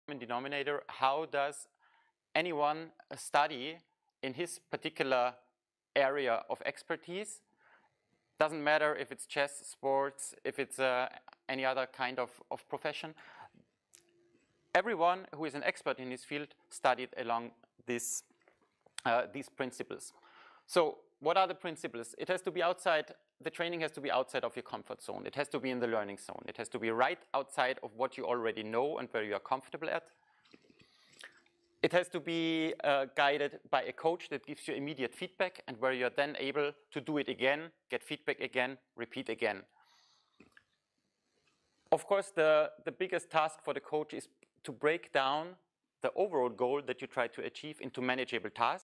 Clemens Jaeger -Supporting children from socio-economic disadvantaged homes with freecoaching
Innovations and Design Research in STEAM Education May 9-10